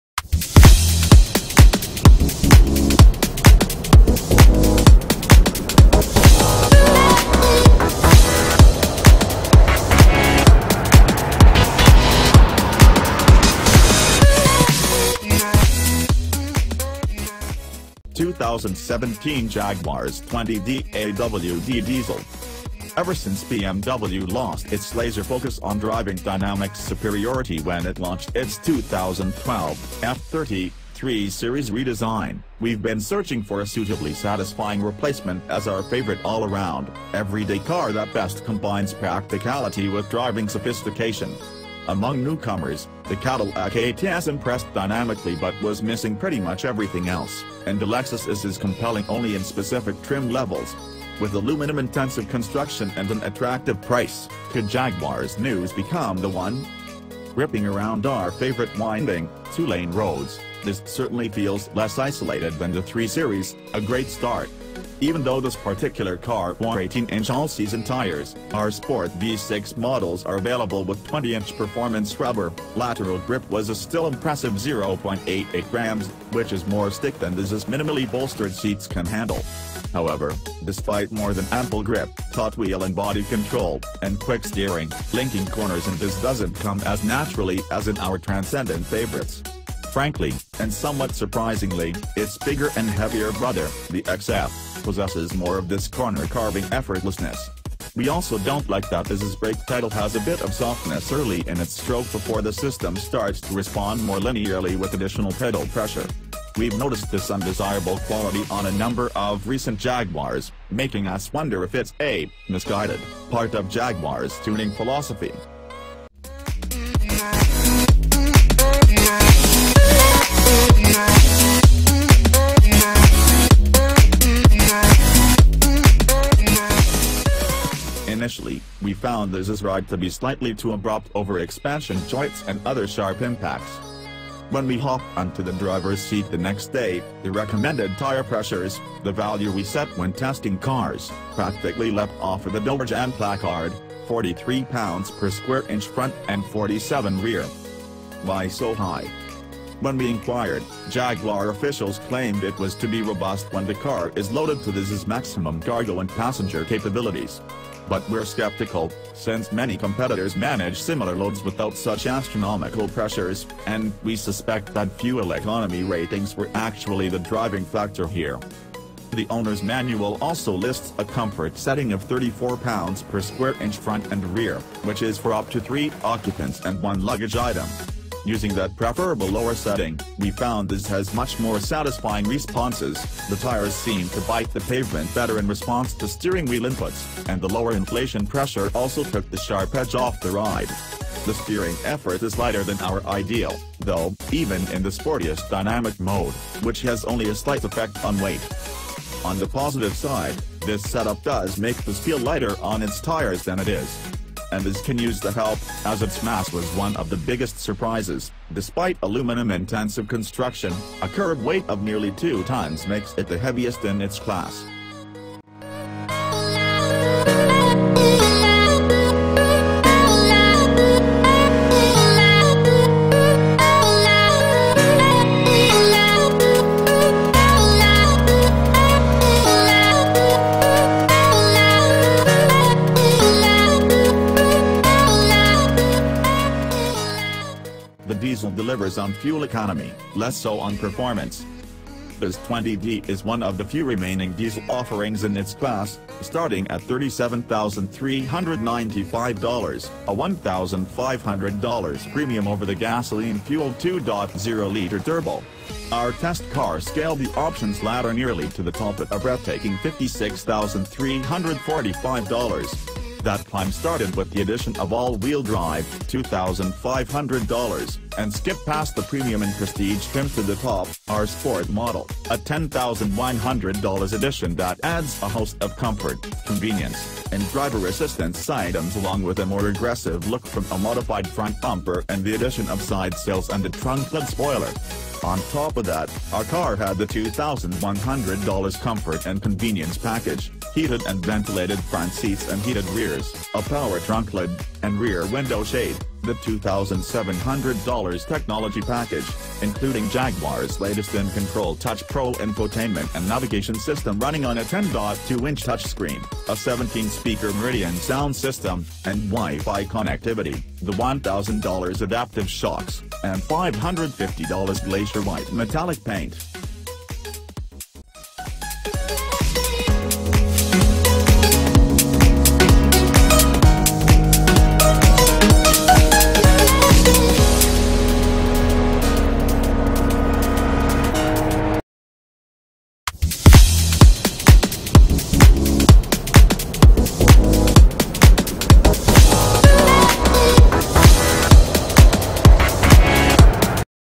HOT NEWS | 2017 Jaguar XE 20d AWD Diesel Review | AutoCar Review
2017 Jaguar XE 20d AWD Diesel Review

Ever since BMW lost its laser focus on driving-dynamics superiority when it launched its 2012 (F30) 3-series redesign, we’ve been searching for a suitably satisfying replacement as our favorite all-around, everyday car that best combines practicality with driving sophistication. Among newcomers, the Cadillac ATS impressed dynamically but was missing pretty much everything else, and the Lexus IS is compelling only in specific trim levels. With aluminum-intensive construction and an attractive price, could Jaguar’s new XE become The One?

Ripping around our favorite winding, two-lane roads, the XE certainly feels less isolated than the 3-series—a great start. Even though this particular car wore 18-inch all-season tires—R-Sport V-6 models are available with 20-inch performance rubber—lateral grip was a still impressive 0.88 g, which is more stick than the XE’s minimally bolstered seats can handle. However, despite more than ample grip, taut wheel and body control, and quick steering, linking corners in the XE doesn’t come as naturally as in our transcendent favorites. Frankly, and somewhat surprisingly, its bigger and heavier brother, the XF, possesses more of this corner-carving effortlessness. We also don’t like that the XE’s brake pedal has a bit of softness early in its stroke before the system starts to respond more linearly with additional pedal pressure. We’ve noticed this undesirable quality on a number of recent Jaguars, making us wonder if it’s a (misguided) part of Jaguar’s tuning philosophy.